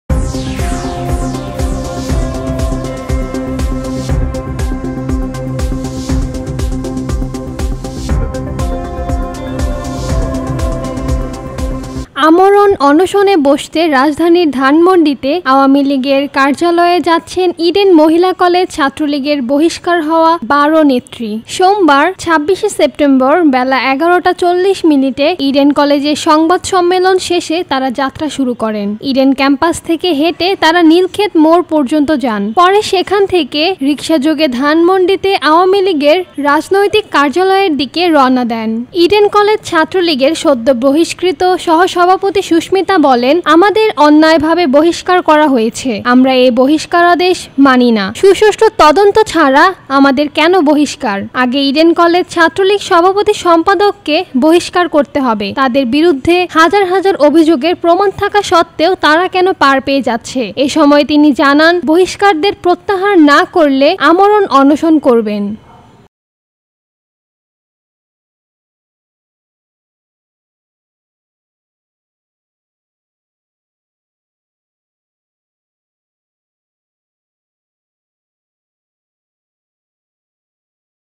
অনশনে বসতে আ’লীগ কার্যালয়ে যাচ্ছেন ইডেন ছাত্রলীগের ১২ নেত্রী। Vtv
আমরণ অনশনে বসতে রাজধানীর ধানমন্ডিতে আওয়ামী লীগের কার্যালয়ে
যাচ্ছেন ইডেন মহিলা কলেজ ছাত্রলীগের বহিষ্কার হওয়া ১২ নেত্রী।
সোমবার (২৬ সেপ্টেম্বর) বেলা ১১টা ৪০ মিনিটে ইডেন কলেজে সংবাদ
সম্মেলন শেষে তারা যাত্রা শুরু করেন।
ইডেন ক্যাম্পাস থেকে হেটে তারা নীলক্ষেত মোড় পর্যন্ত যান। পরে সেখান
থেকে রিকশাযোগে ধানমন্ডিতে আওয়ামী লীগের রাজনৈতিক কার্যালয়ের দিকে
রওয়ানা দেন।
ইডেন কলেজ ছাত্রলীগের সদ্য বহিষ্কৃত সহ-সভাপতি সুস্মিতা বাড়ৈ বলেন,
আমাদের অন্যায়ভাবে বহিষ্কার করা হয়েছে। আমরা এই বহিষ্কারাদেশ মানি
না। সুষ্ঠু তদন্ত ছাড়া আমাদের কেন বহিষ্কার? আগে ইডেন কলেজ ছাত্রলীগ
সভাপতি-সম্পাদকে বহিষ্কার করতে হবে। তাদের বিরুদ্ধে হাজার হাজার
অভিযোগের প্রমাণ থাকা সত্ত্বে তারা কেন পার পেয়ে যাচ্ছে।
এসময় তিনি জানান, বহিষ্কারাদেশ প্রত্যাহার না করলে আমরণ অনশন
করবেন।
This channel is a pure entertainment channel which will be broadcasting current affairs , Historical Documentary , Entertainments and many more.